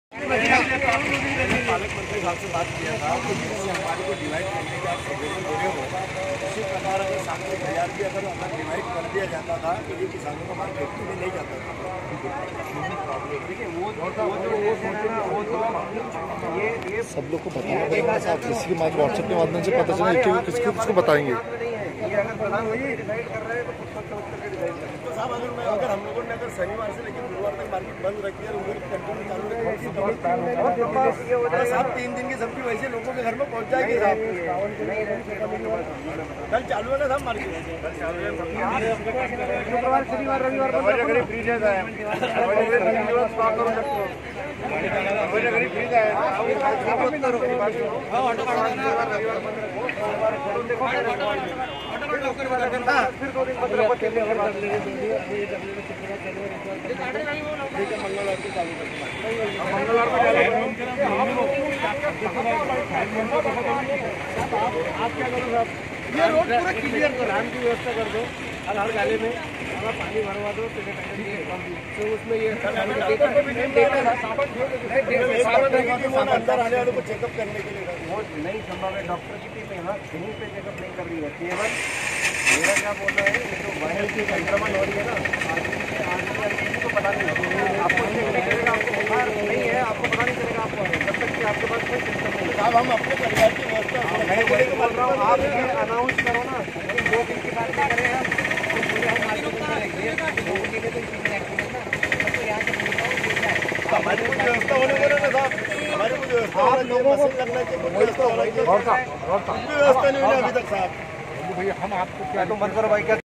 President Nandu Gour discussed to IPS Neelotpal Saheb regarding APMC Sabji Market 25/03/20
Farmers, Retailers, Employees aur Adtiya ki safety k liye maine IPS Officer Mr. Neelotpal Sahab, APMC MARKET Secratary Rajesh Bhusaari Ji, Kalmana Police Inspector Vishwanath Chouhan Saheb k saamne sabji Market ki poori jaankari di aur poore Market ko sanetiser karne k liye kahan aur 6 din k bajaaye ab market 3 din band rahega aisa nirnaye liya gaya